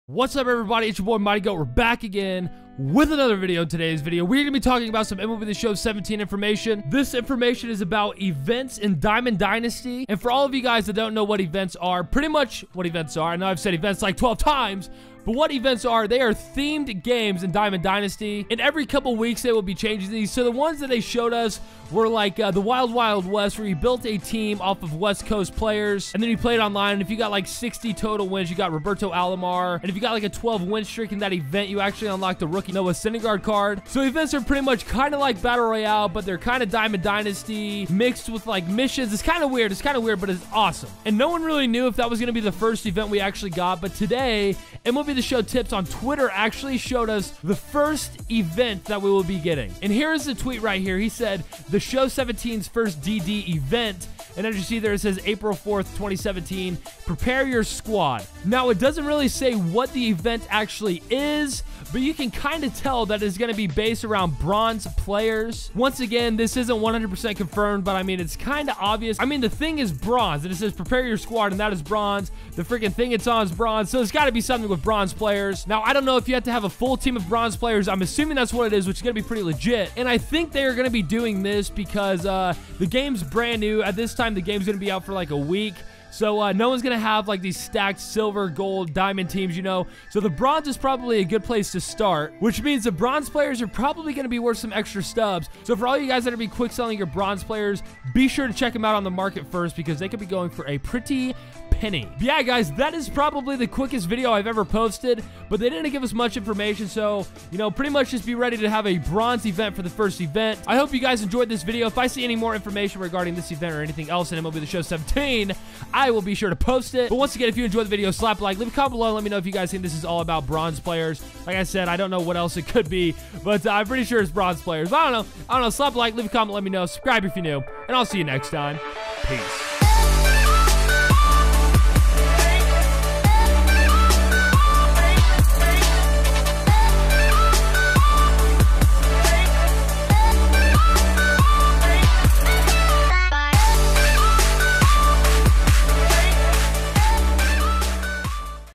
NEW MLB THE SHOW 17 DIAMOND DYNASTY INFO! The First Ever Event?!
In This MLB The Show 17 video, I talk about brand new information reguarding MLB The Show 17 diamond dynasty! This information gives us a hint as to what the first event will be in MLB the Show 17! I cannot wait to play this game!

Snap Chat - @itsmightygoat